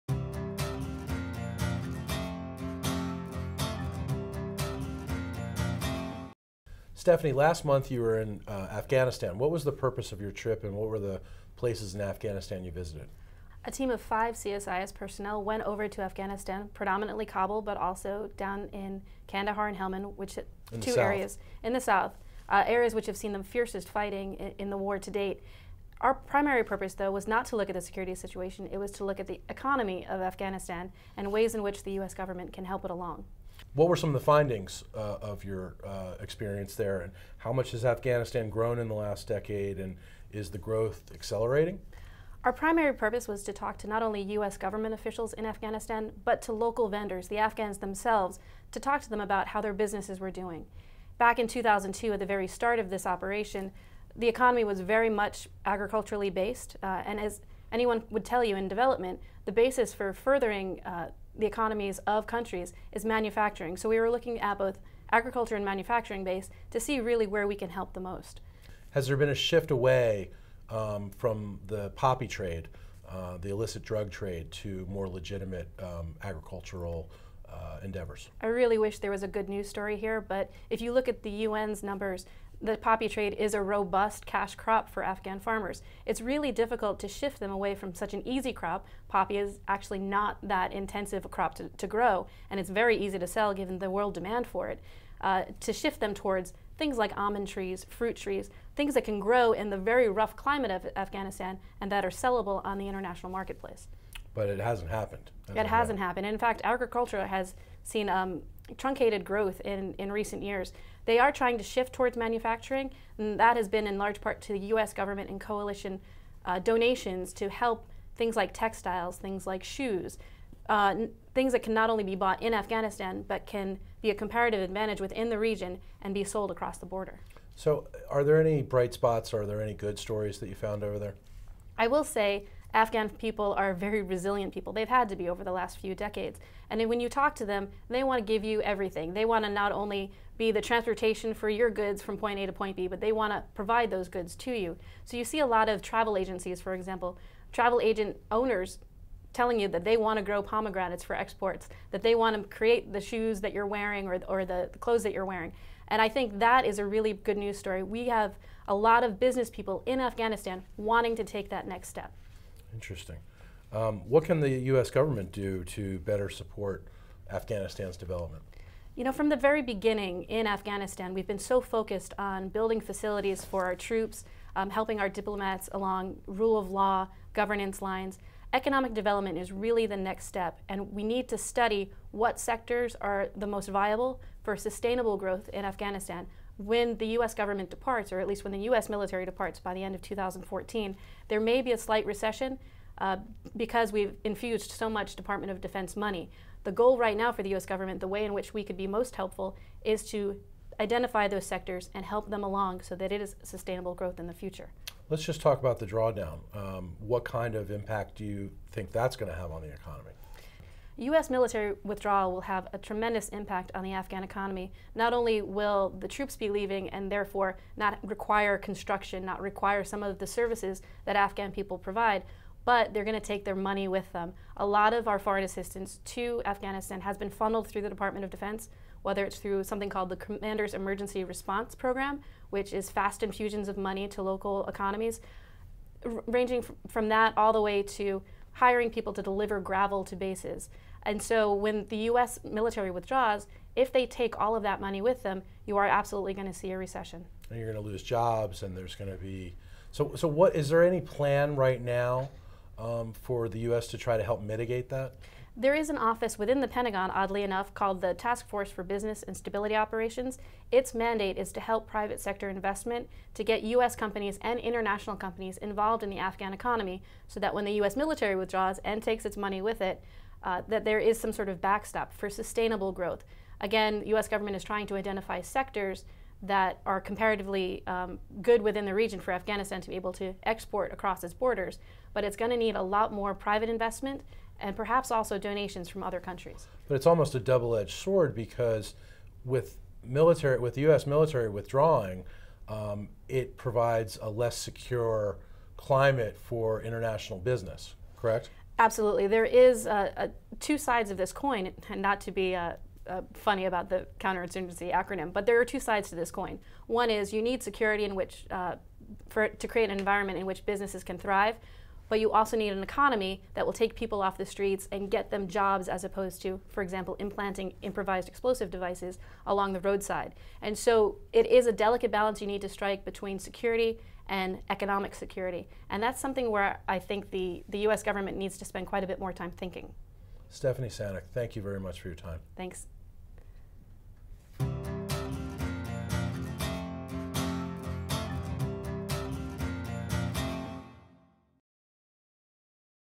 Afghanistan's Economic Prospect
Stephanie Sanok discusses her recent trip to Afghanistan and shares observations on the current policy landscape following the President's announcement of an accelerated withdrawal timetable and what still needs to be done.